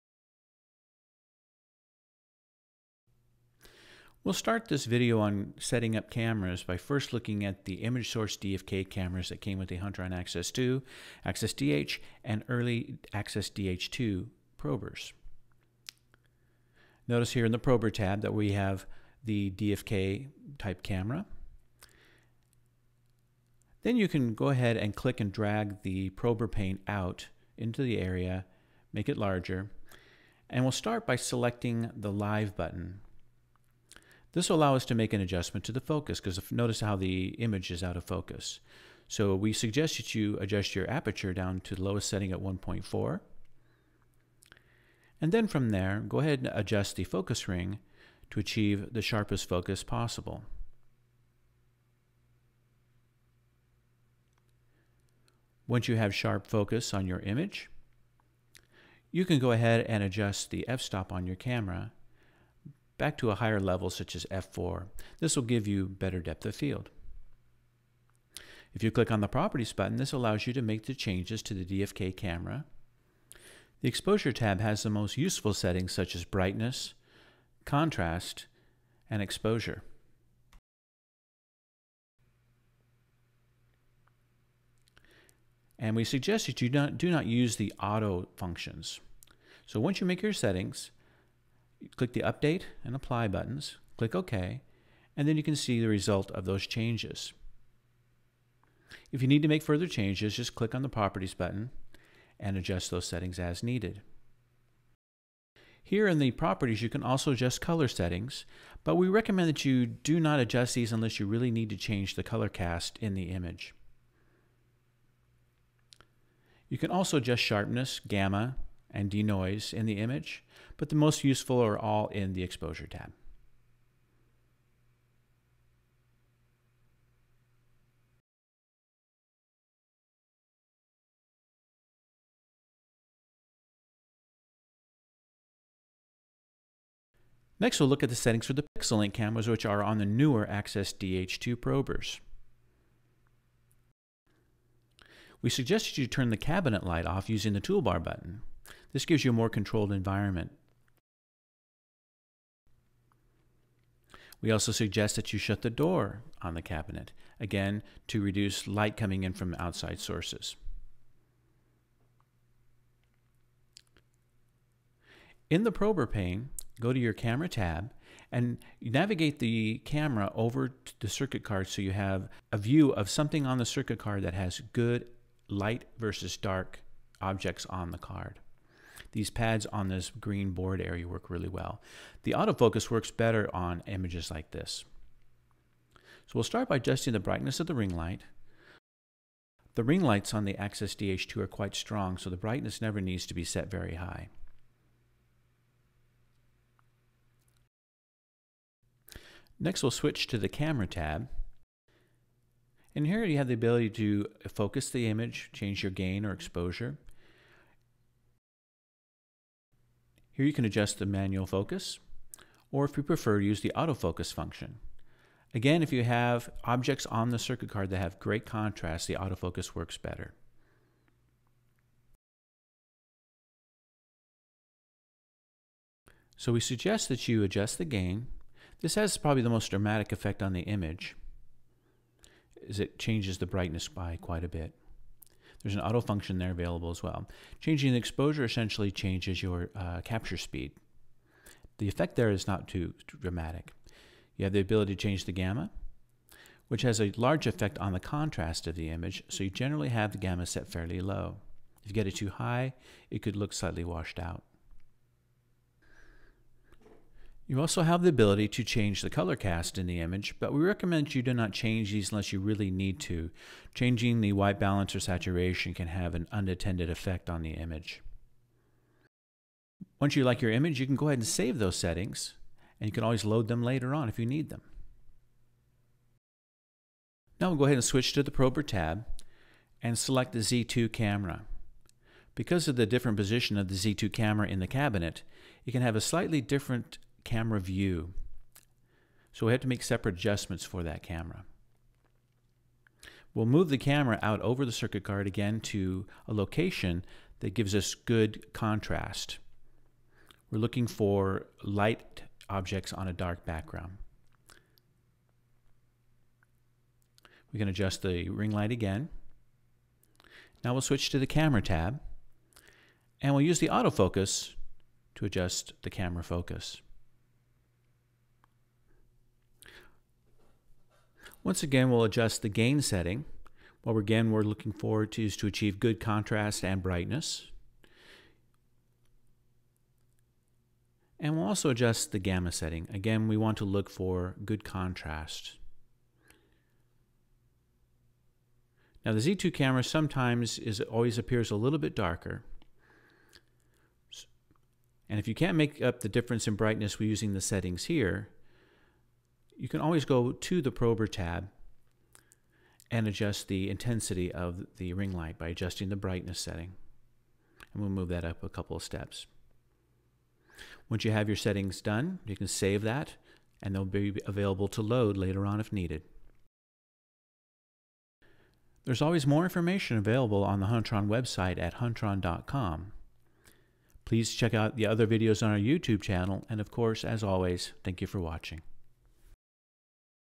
Camera Setup for Access 2, Access DH and DH2 Probers
This short video discusses configuring the settings for the Image Source DFK cameras used on the Access 2, Access DH and pre-2022 Access DH2 Probers from Huntron. Also covered is the setup for the Pixelink cameras used on the Access DH2 starting in 2022.